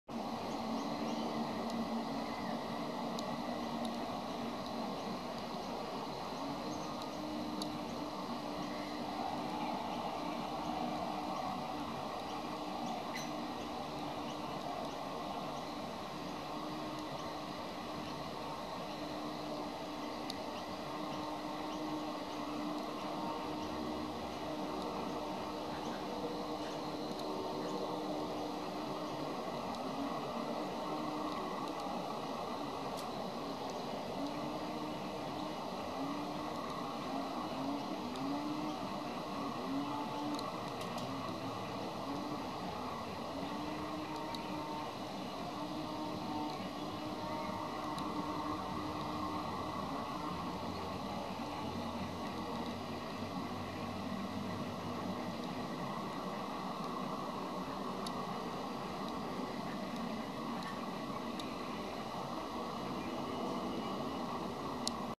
Loving Lorikeets
A pair of loving rainbow lorikeets on our veranda 101015.